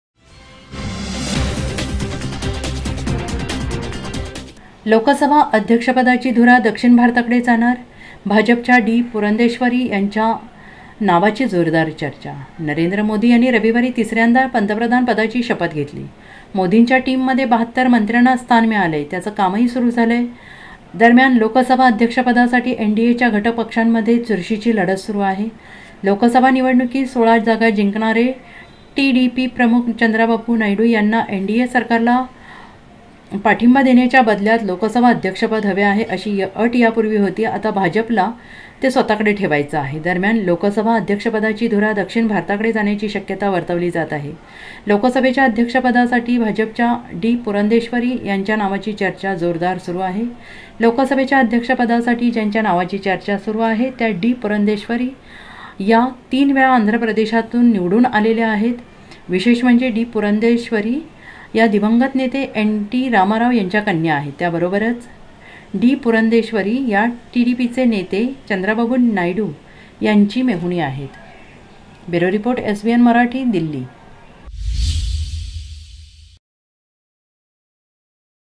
DELHI | लोकसभा अध्यक्षपदाची धुरा दक्षिण भारताकडे जाणार?, डी. पुरंदेश्वरी यांच्या नावाची जोरदार चर्चा
लोकसभा अध्यक्षपदाची धुरा दक्षिण भारताकडे जाणार?, भाजपच्या डी. पुरंदेश्वरी यांच्या नावाची जोरदार चर्चा.